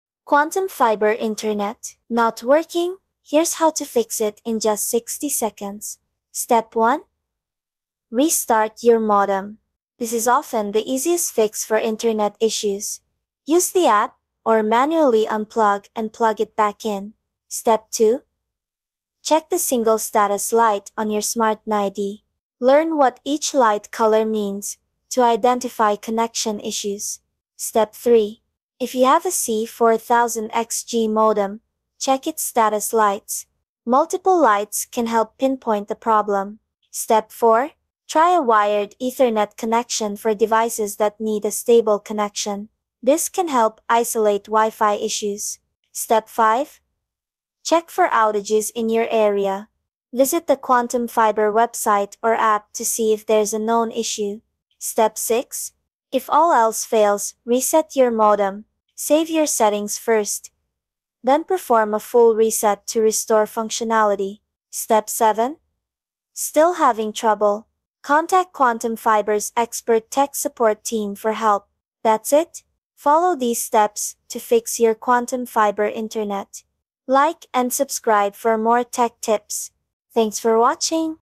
QUANTUM FIBER INTERNET NOT WORKING | HOW TO EASILY FIX! FULL GUIDE!
Experiencing Quantum Fiber internet issues? This video provides a comprehensive guide on how to easily fix common problems. We'll walk you through troubleshooting steps, from checking connections to resetting your modem and router. Get your Quantum Fiber back up and running quickly with this full guide!

CHAPTERS:
0:00 INTRO
0:04 TUTORIAL